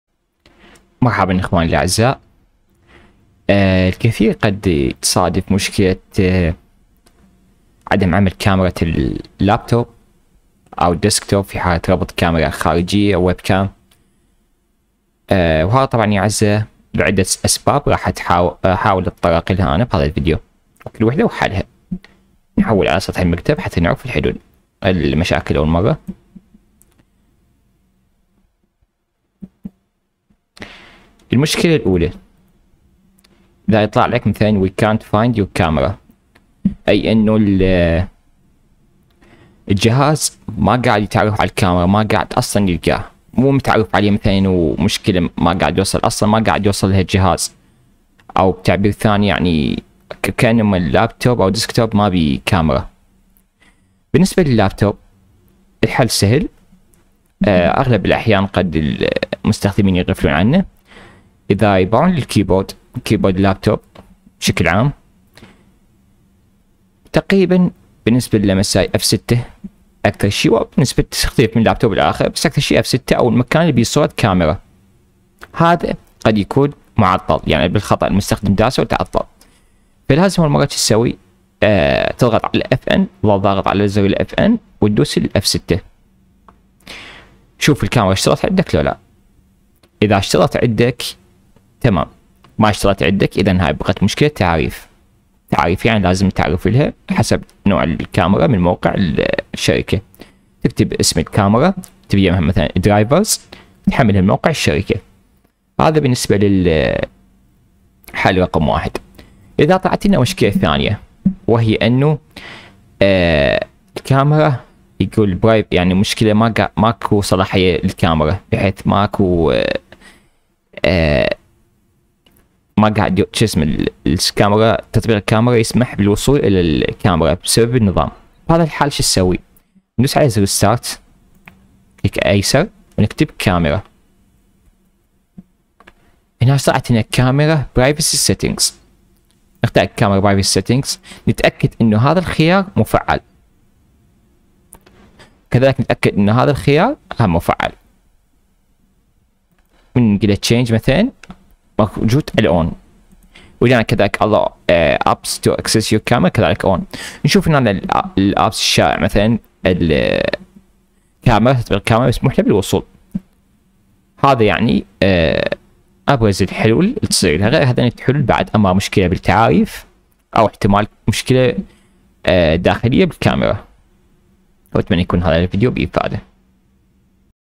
مشكلة عدم عمل كاميرا الابتوب او ويب كام خارجية على الحهاز وابرز الحلول
مرحبا اخواني الاعزاء بهذا الفديو حبيت اوضح كيفية حل ابرز المشاكل المتعلقة عند ربط كاميرا خارجية او كاميرا اللابتوب نفسها